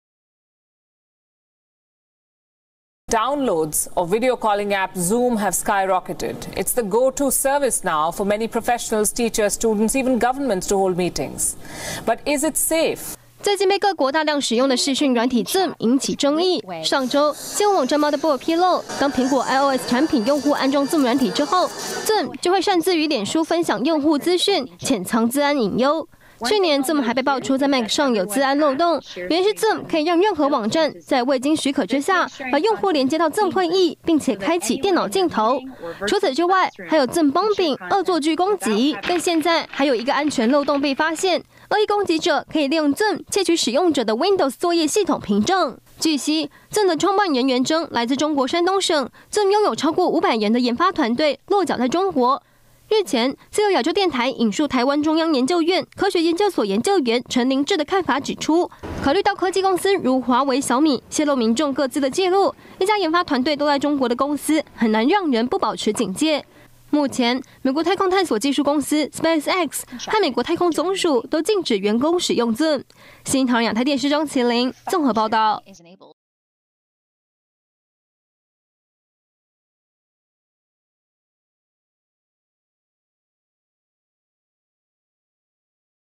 Zoom中國研發引憂慮 NASA和SpaceX禁用｜新唐人亞太電視｜20200405
中共病毒疫情嚴峻，全球約半數人口行動受限。許多企業使用視訊軟體「Zoom」來線上會議運作，也有學校用「Zoom」線上教學。不過，「Zoom」被發現資安漏洞。